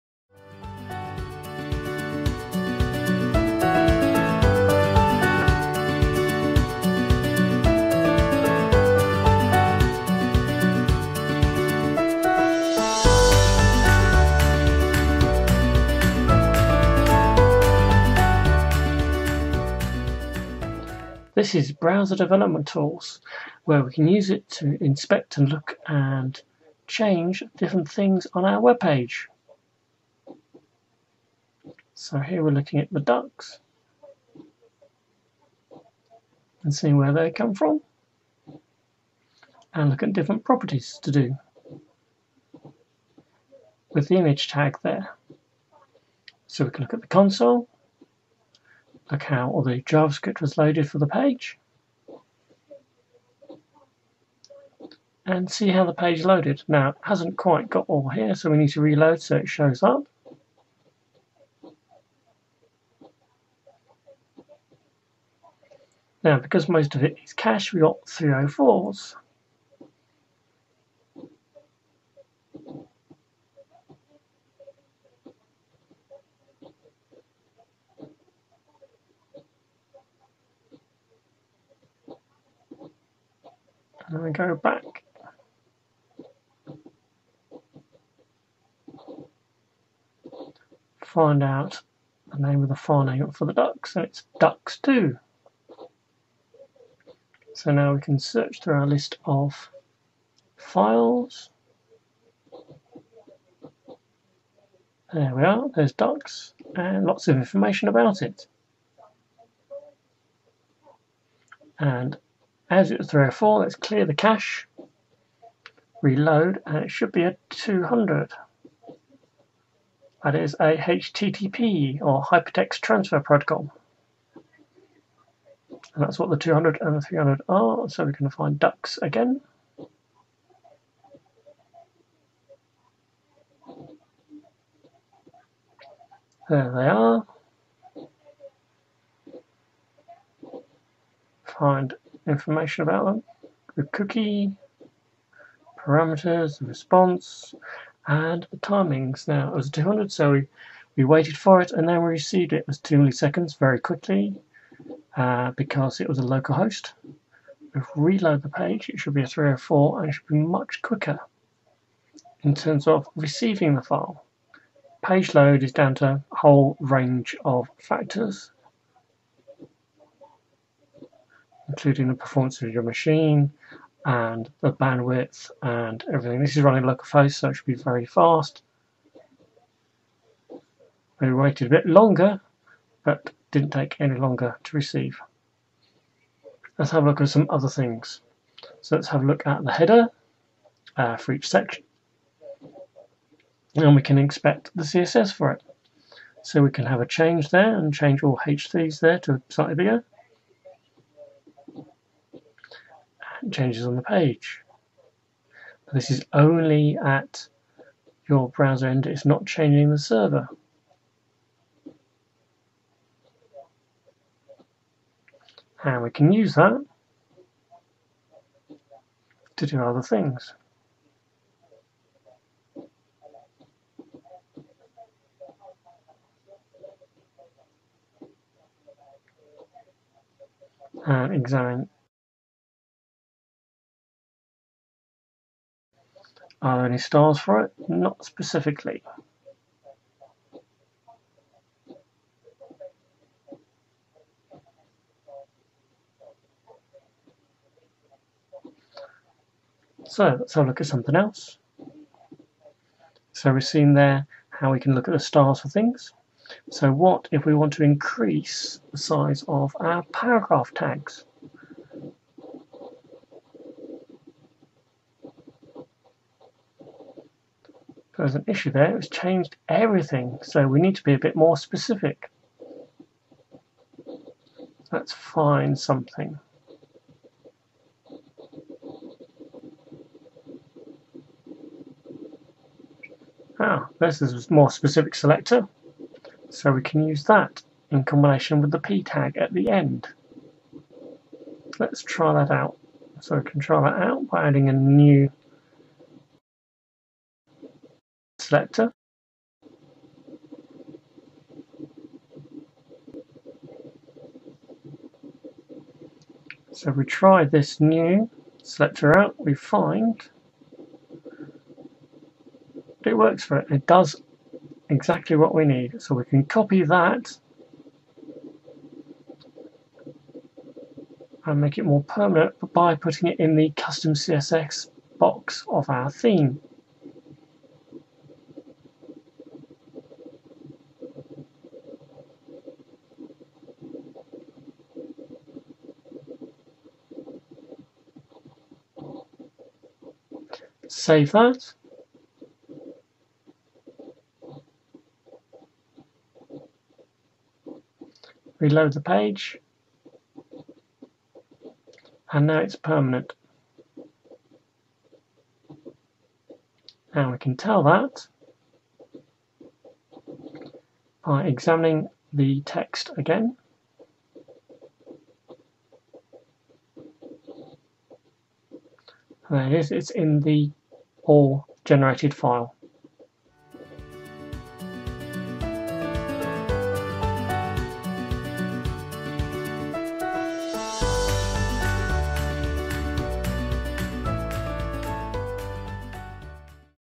Theme developer tools
An overview of the browser developer tools available for Theme designers.